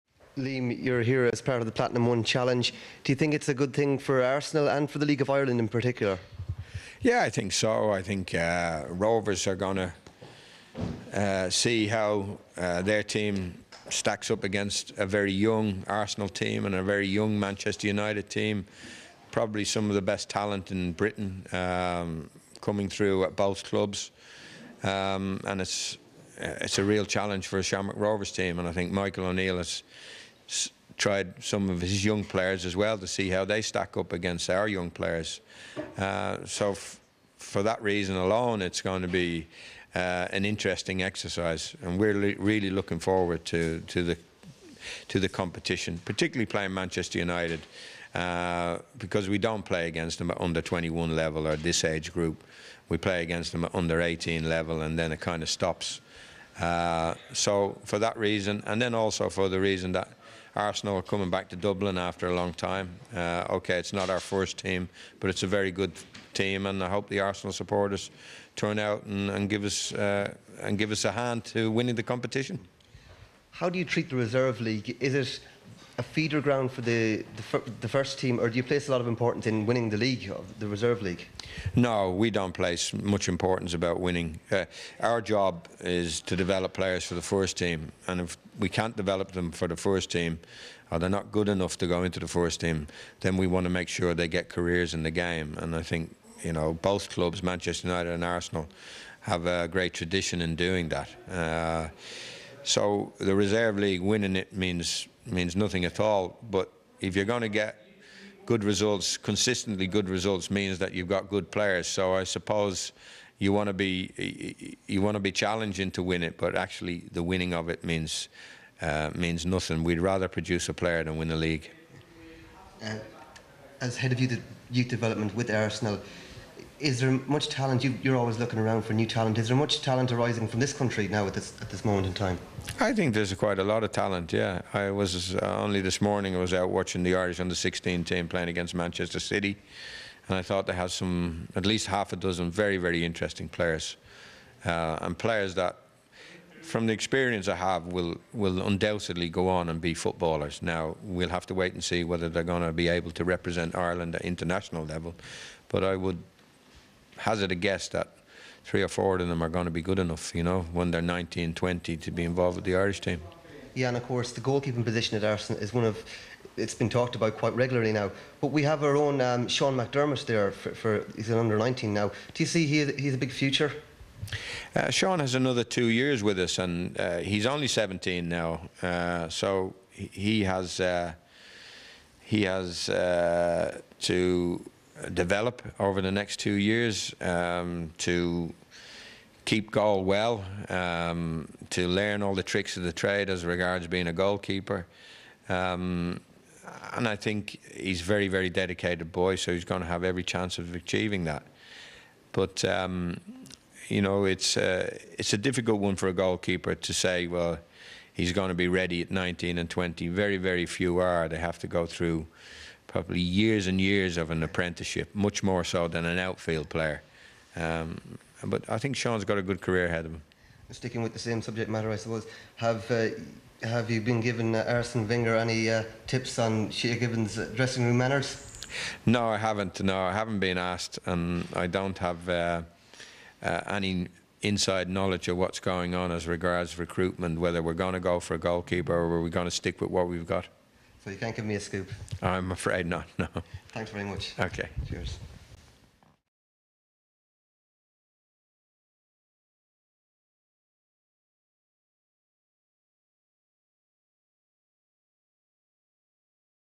Liam Brady - Arsenal Reserves
Extratime.ie reporter Jody Moylan chats to Liam Brady. Filmed by Peter O'Doherty.